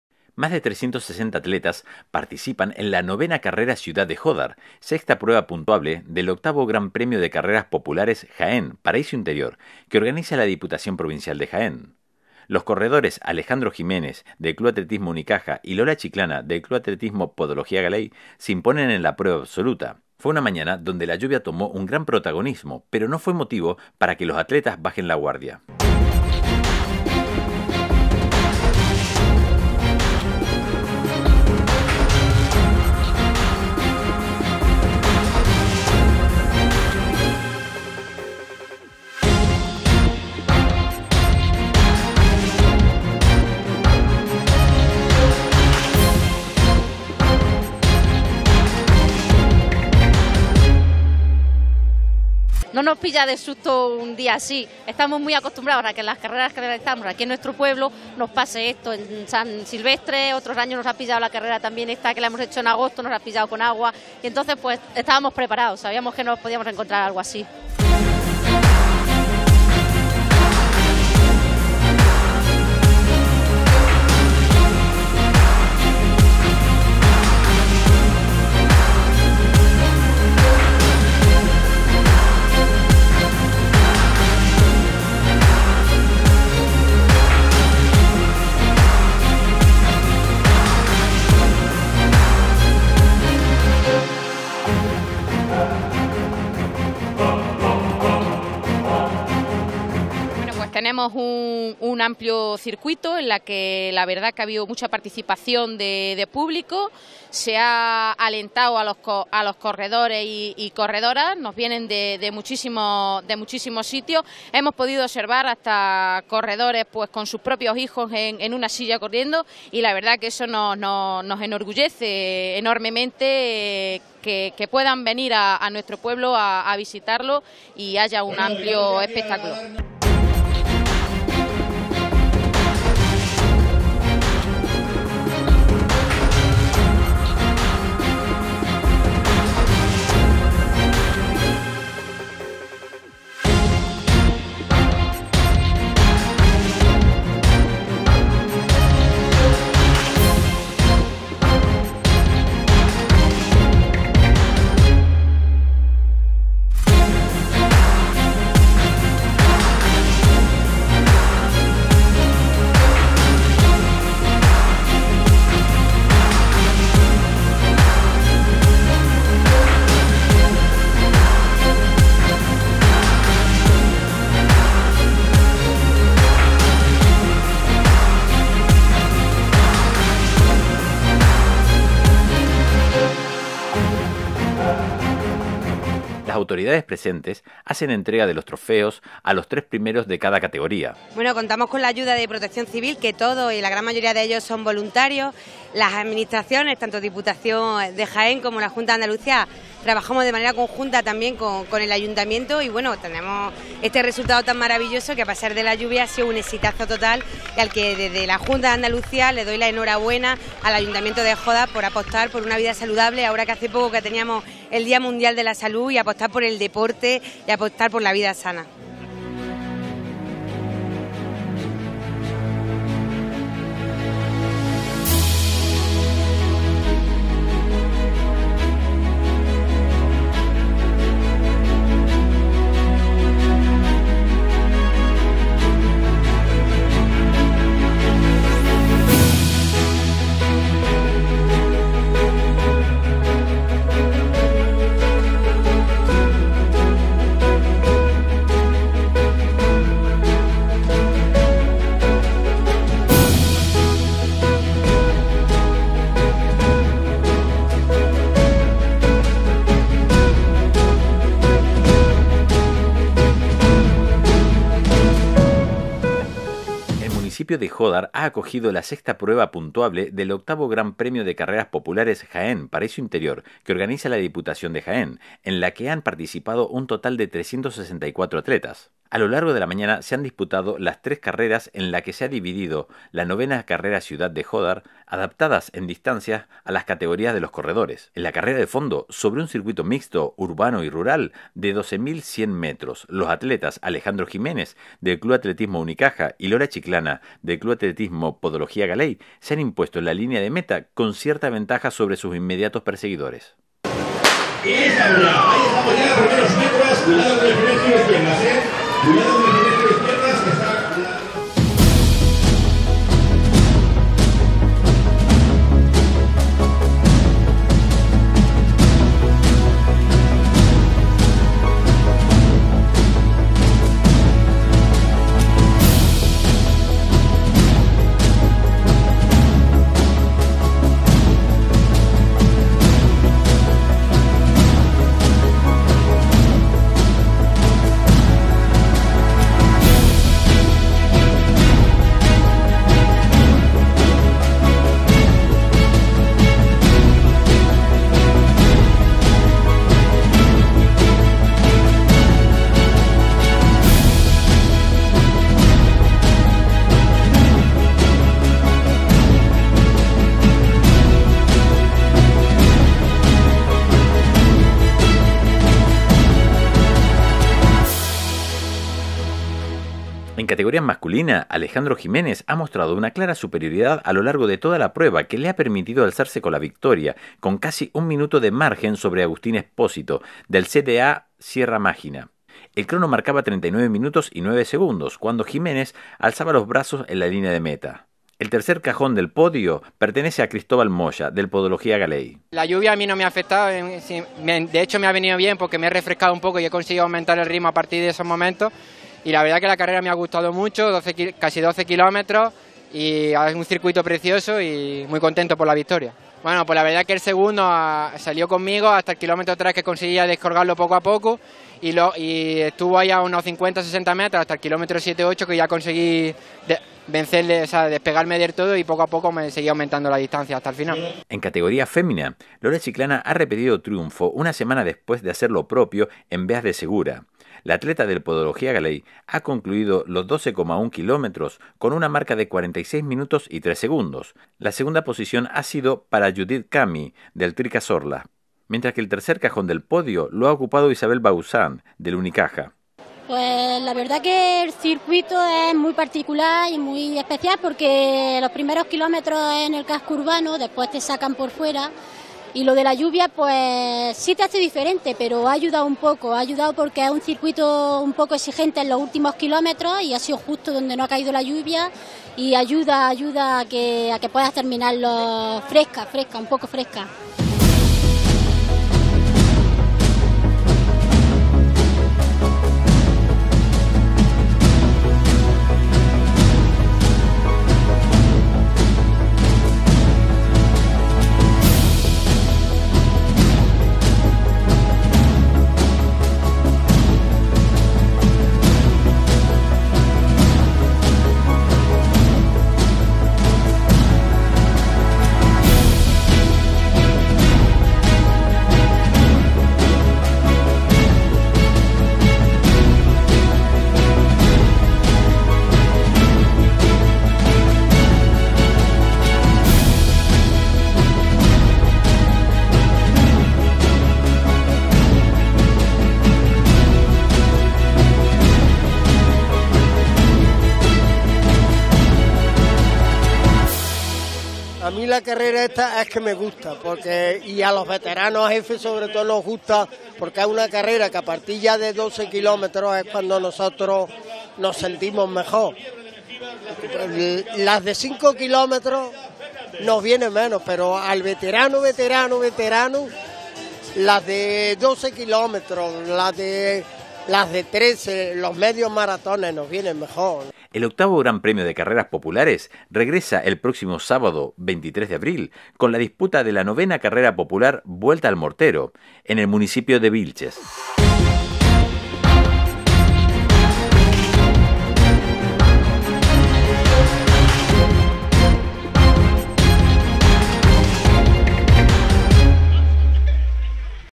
IX Carrera Urbana Ciudad de Jódar 2016
El municipio de Jódar ha acogido la sexta prueba puntuable del VIII Gran Premio de Carreras Populares “Jaén, paraíso interior”, que organiza la Diputación y en la que han participado un total de 364 atletas. A lo largo de esa mañana se han disputado las tres carreras en las que se ha dividido la IX Carrera Ciudad de Jódar, adaptadas en distancia a las categorías de los corredores. En la carrera de fondo, sobre un circuito urbano y rústico de 12.100 metros, los atletas Alejandro Jiménez, del Club Atletismo Unicaja, y Lola Chiclana, del Club Atletismo Podología Galey, se han impuesto en la línea de meta con cierta ventaja sobre sus inmediatos perseguidores.

En categoría masculina, Alejandro Jiménez, ha mostrado una superioridad a lo largo de toda la prueba que le ha permitido alzarse con la victoria con casi un minuto de margen sobre Agustín Expósito, del CDA Sierra Mágina. El crono marcaba 39 minutos y 9 segundos cuando Jiménez alzaba los brazos en la línea de meta. En tercera posición ha entrado Cristóbal Moya, del Podología Galey.

En categoría femenina, Lola Chiclana ha repetido triunfo una semana después de hacer lo propio en Beas de Segura. La atleta del Podología Galey ha concluido los 12,1 kilómetros con una marca de 46 minutos y 3 segundos. La segunda posición ha sido para Judit Cami, del Tricazorla, mientras que el último cajón del podio lo ha ocupado Isabel Bausán, del Unicaja.

El VIII Gran Premio de Carreras Populares regresa el próximo sábado, 23 de abril, con la disputa de la IX Carrera Popular “Vuelta al Mortero”, en el municipio de Vilches.